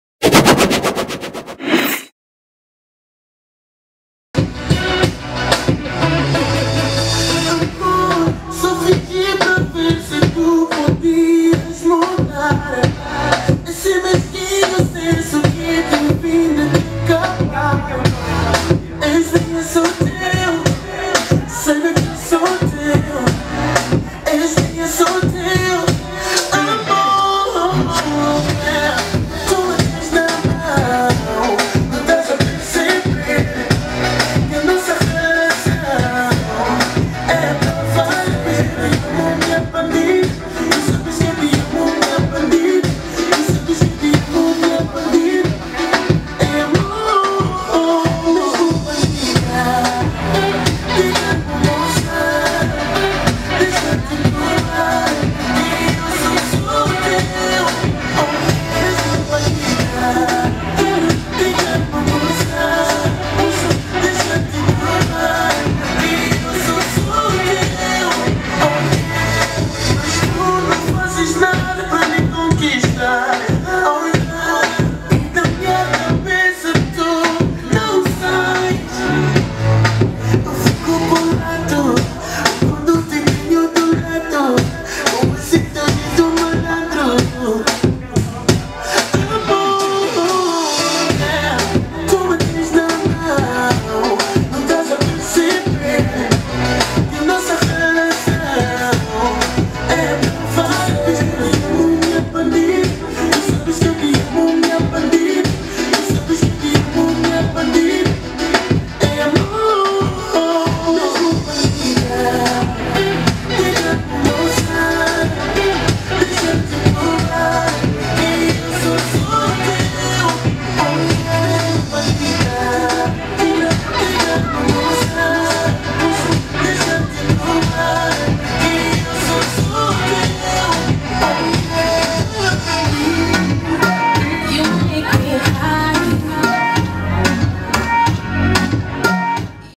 Albir Rojas & Lital Weiss dance Kizomba 2
Albir Rojas & Lital Weiss dance Kizomba in Studio 13, Israel.